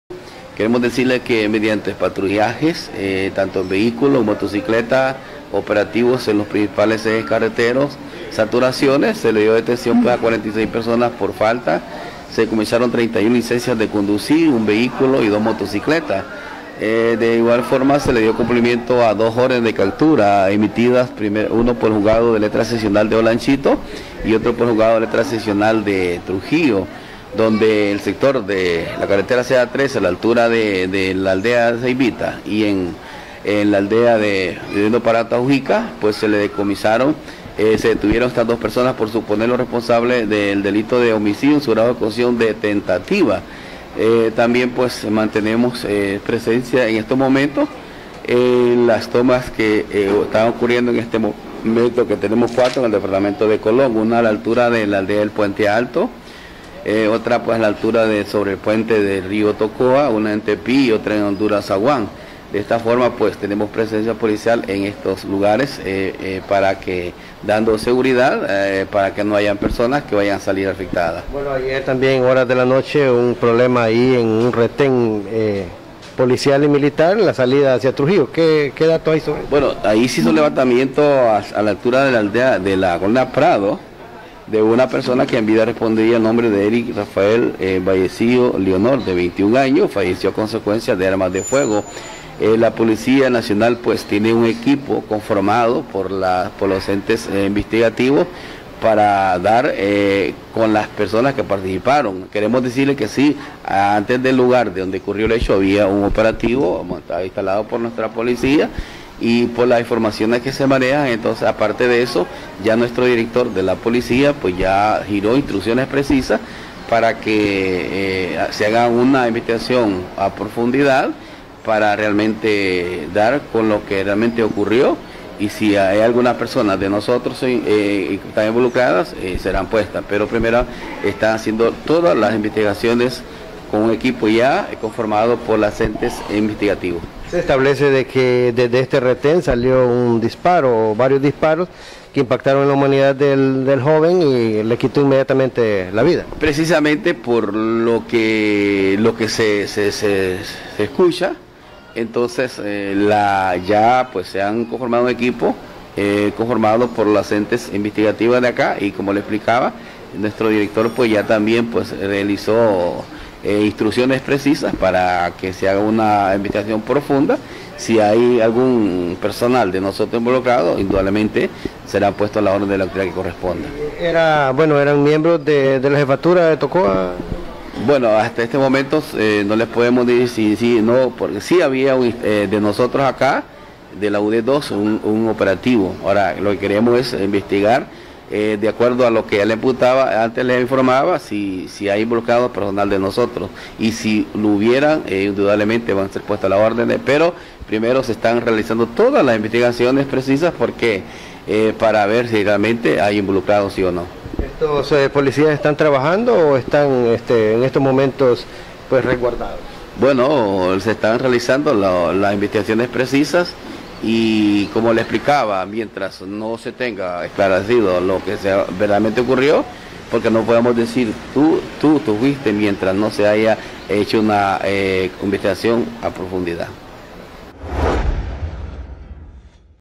Policía nacional dispara y da muerte a Erick Vallecillo.
Por no detenerse en un retén en Tocoa policía nacional le dispara a joven quitándole la vida.